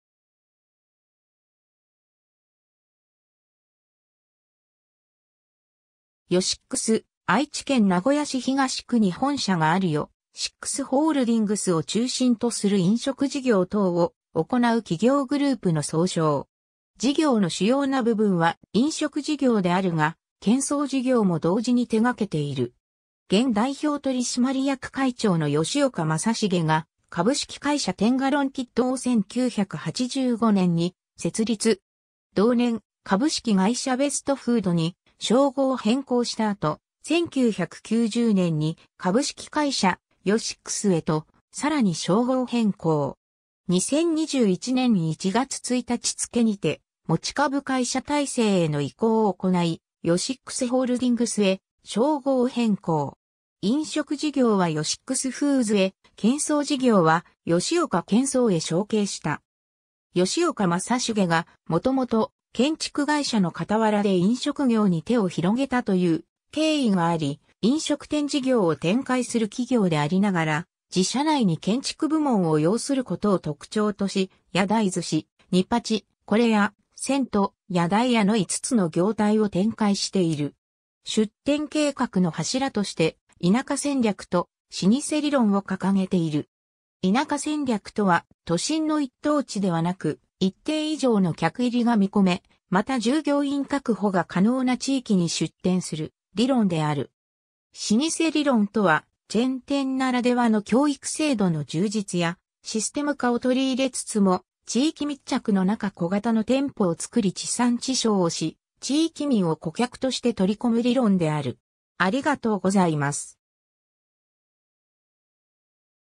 ヨシックス
ヨシックス、愛知県名古屋市東区に本社があるヨシックスホールディングスを中心とする飲食事業等を行う企業グループの総称。
事業の主要な部分は飲食事業であるが、建装事業も同時に手がけている。
 現代表取締役会長の吉岡昌成が株式会社テンガロンキッドを1985年に設立。
同年、株式会社ベストフードに商号変更した後、1990年に株式会社ヨシックスへとさらに商号変更。
2021 年１月１日付にて持株会社体制への移行を行いヨシックスホールディングスへ商号変更。
飲食事業はヨシックスフーズへ建装事業は吉岡建装へ承継した。
 吉岡昌成が元々、建築会社の傍らで飲食業に手を広げたという経緯があり飲食店事業を展開する企業でありながら自社内に建築部門を擁することを特徴とし、「や台ずし(やだいずし)」、「ニパチ」、「これや」、「せんと」、「や台や(やたいや)」の5つの業態を展開している。
 出店計画の柱として「田舎戦略」と「老舗理論」を掲げている。
「田舎戦略」とは都心の一等地ではなく一定以上の客入りが見込め、また従業員確保が可能な地域に出店する理論である。
「老舗理論」とはチェーン店ならではの教育制度の充実や、システム化をとりいれつつも地域密着の中小型の店舗をつくり地産地消をし、地域民を顧客としてとりこむ理論である。